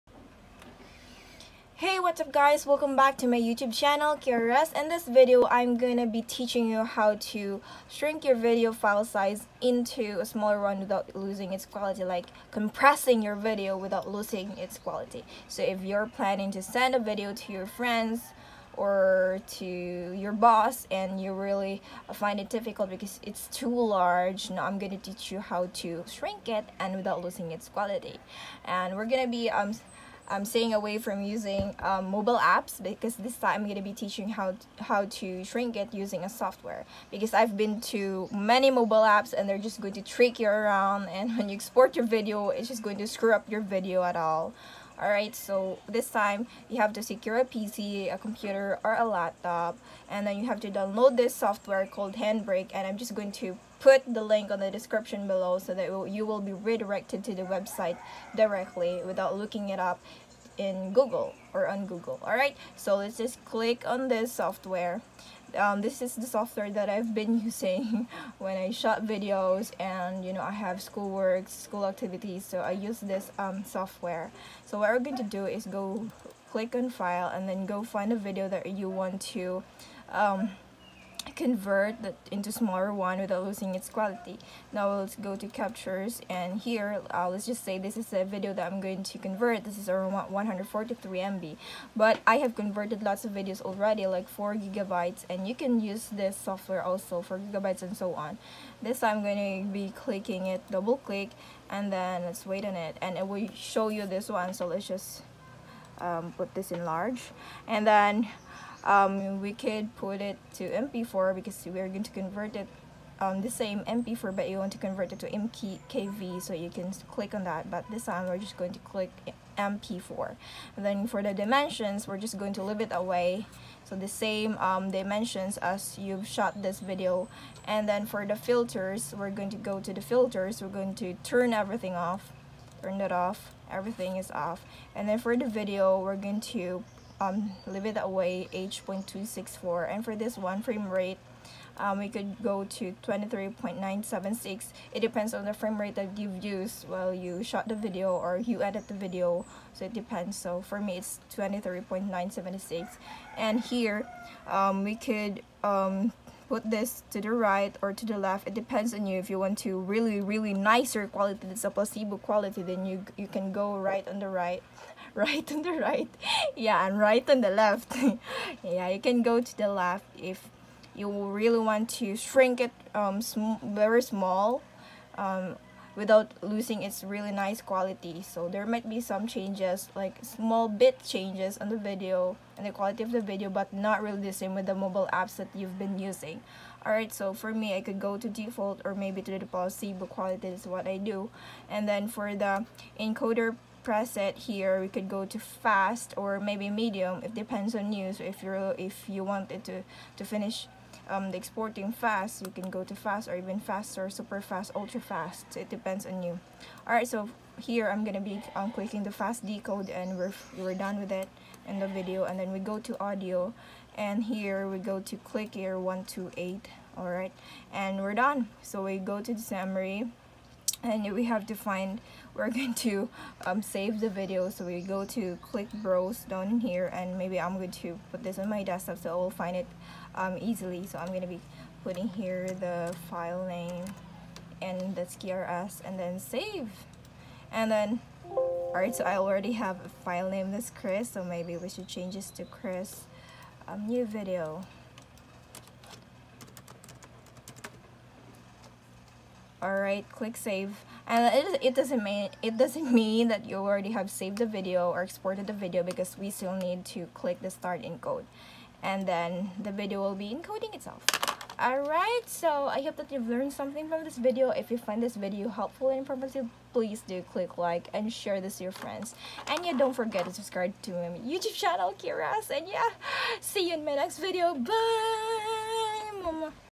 How to Compress a Video File Size WITHOUT Losing Quality 2025
In this video, I’ll show you how to compress a video file size without losing quality in 2025. I’ll walk you through simple and effective methods using online tools and software, so you can reduce file size while keeping your video’s clarity. Watch to learn how to do it!